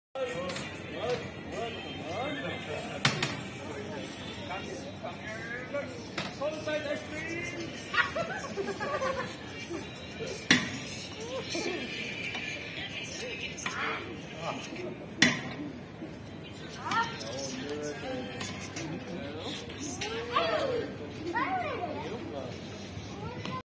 Turkish Ice Cream Tricks..In Qatar. 🇶🇦
Fisrt I saw this,Turkish Ice cream tricks doing by a Turkish guy who was in Souq Waqif in Qatar. So i thought..even it's a small video, I will share with you,my FRIENDS..